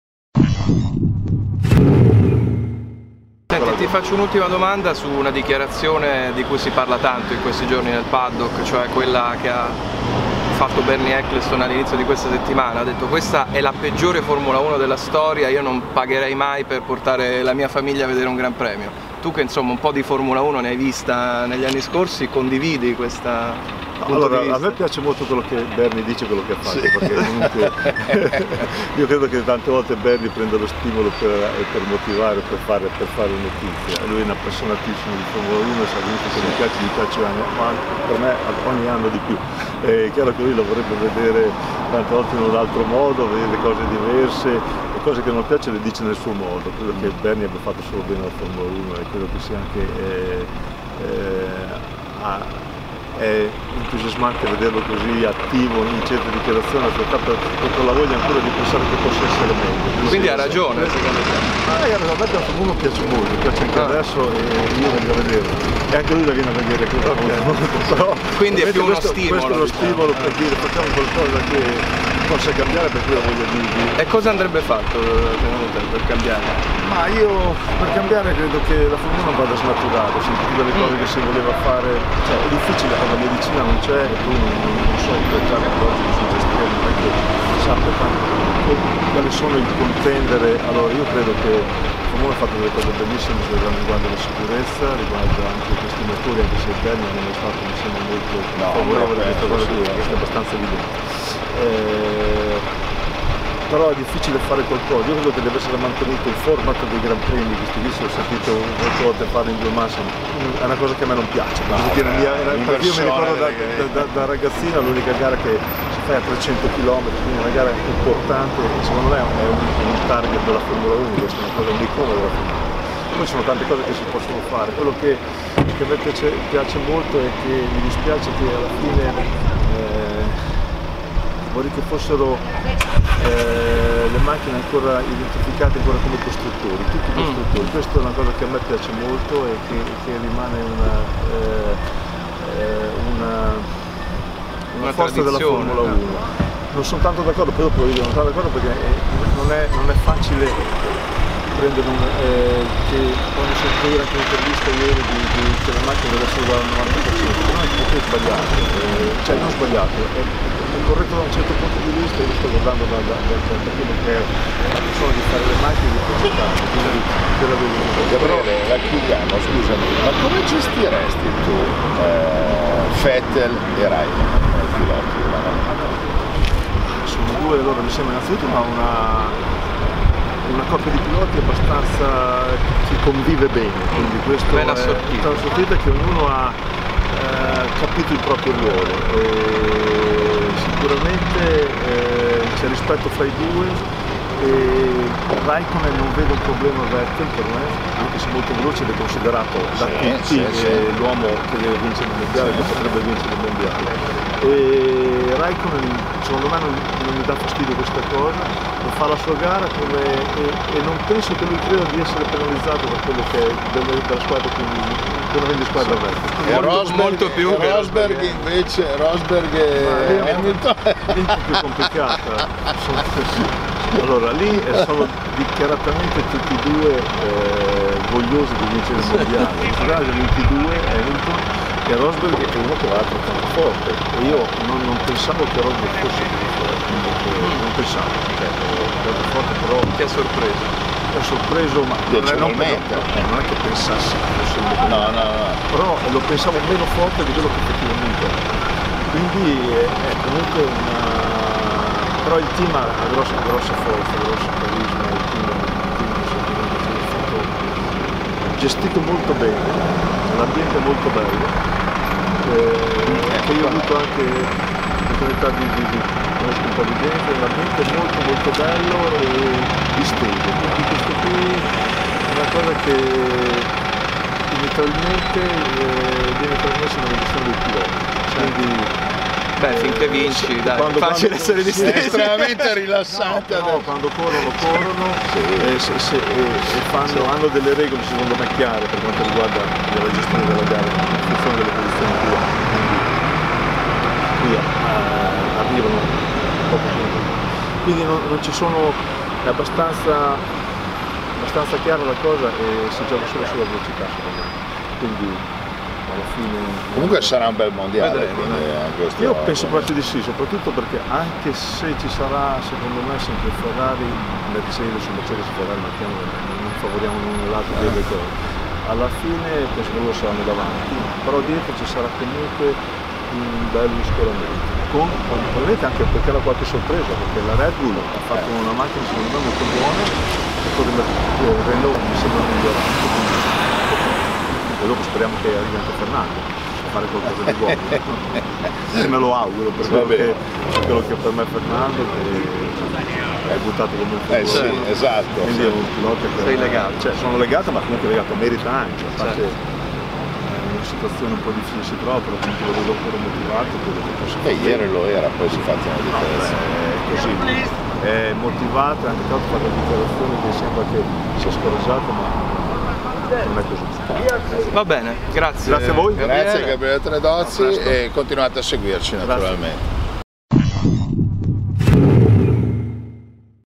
«Vettel-Raikkonen? Ottima coppia»
Concludiamo l'intervista con Gabriele Tredozi, storico direttore tecnico Minardi, dal circuito di Barcellona: si parla di regolamenti, di gestione dei piloti e di rapporti di forza tra le squadre